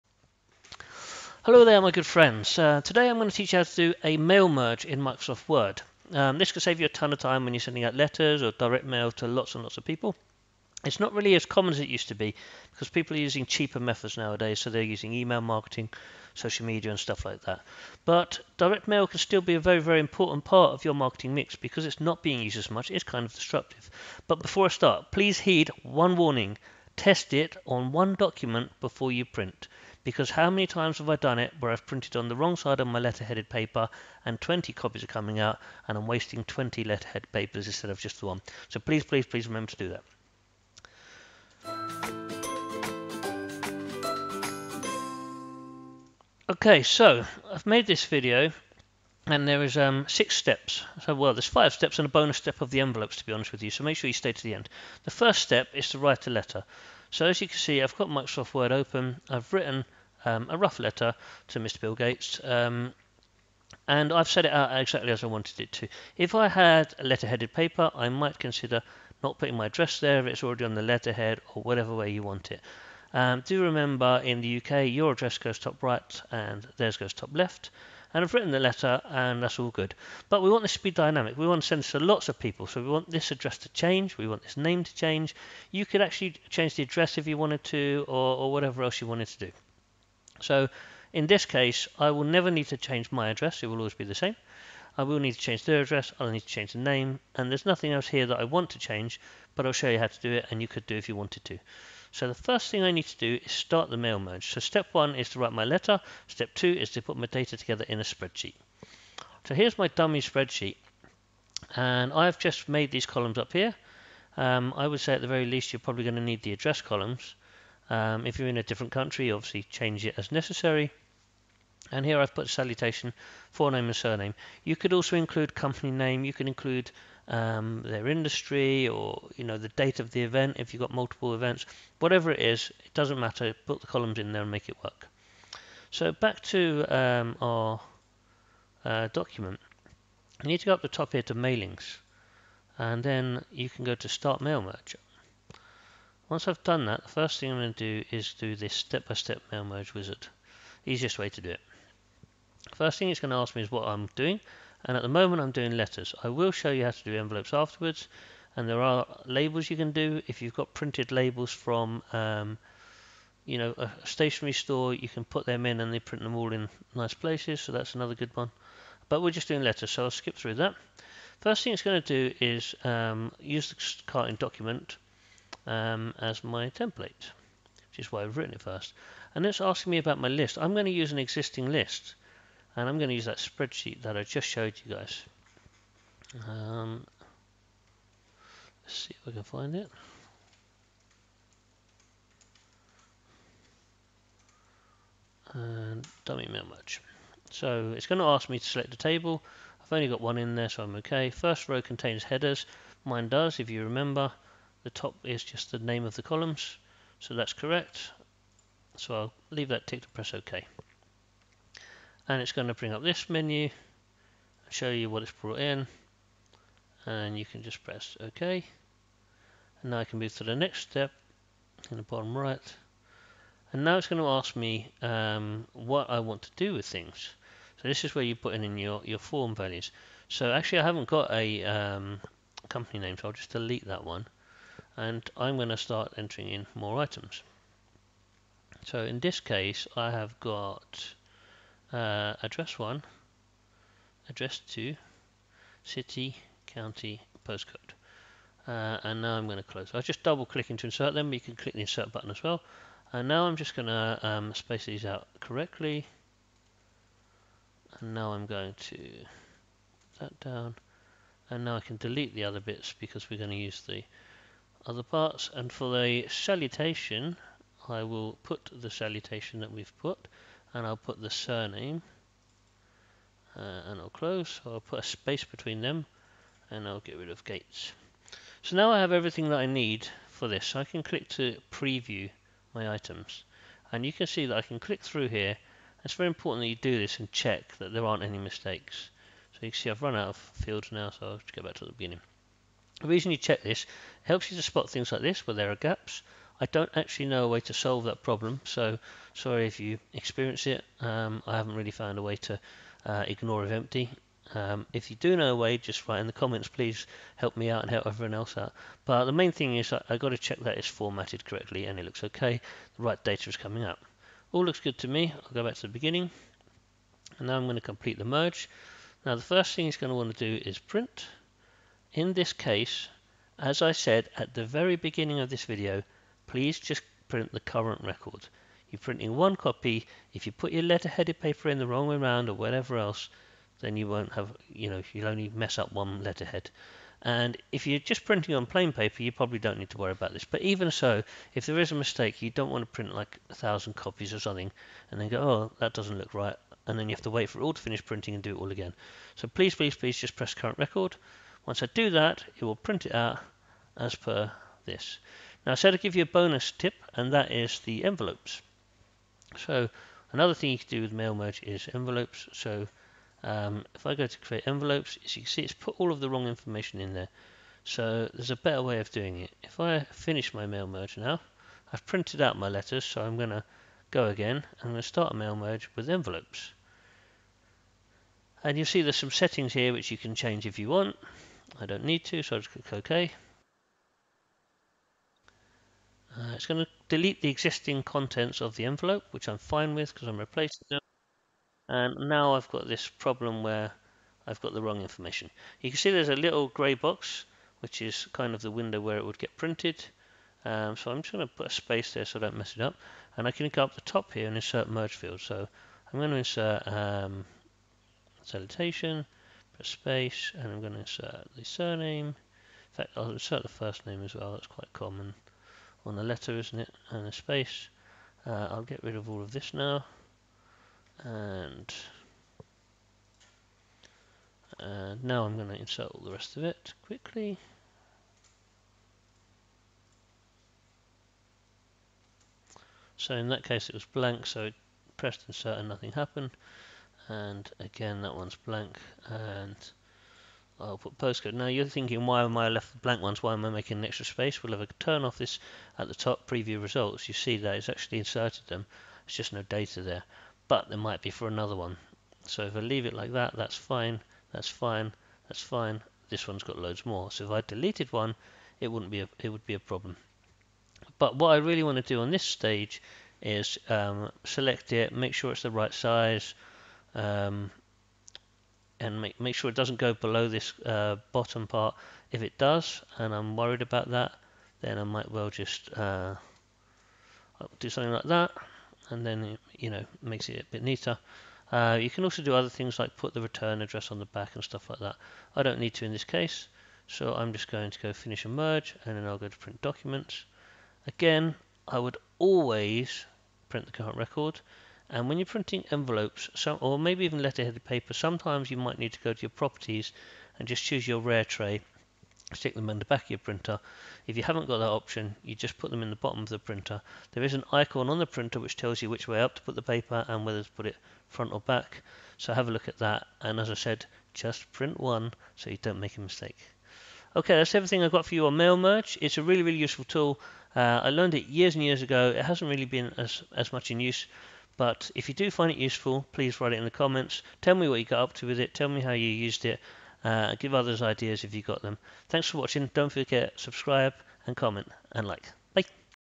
How to do a MailMerge in Microsoft Word
Ever wanted to send out a load of letters?  Don't really want to type out each address for each letter?  Really don't want to hand write a load of envelopes?  Then mail merge is what you are looking for.

I haven't got very neat writing, so even on the handwritten signature parts I download a handwriting font.

This is a great way to perform direct marketing to other business owners, write a load of invites to a party or send out wedding invites.

Whatever you do, mail merge can save you time!